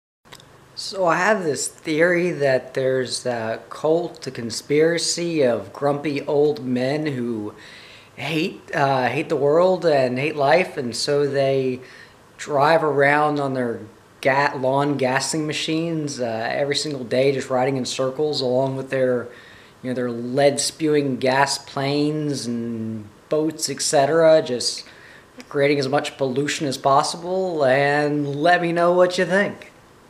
Polluters, Zombies, VooDoo, Dark Arts (See Notes Below)
There are cults of people who use voodoo, brainwashing tactics to torment and torture this can include noise pollution, air pollution and mental pollution. These people are likely possessed by demons (agents of the dark) from their exposure to gas, poison food, chemicals etc. They are polluting themselves and their souls and are disobeying God, that is why the live in misery; It is a natural way of purification, don't let them torment you remember that they are suffering (punishing themselves) for their own sins and God is in control. Voodoo is a false religion and is forbidden by Jehovah.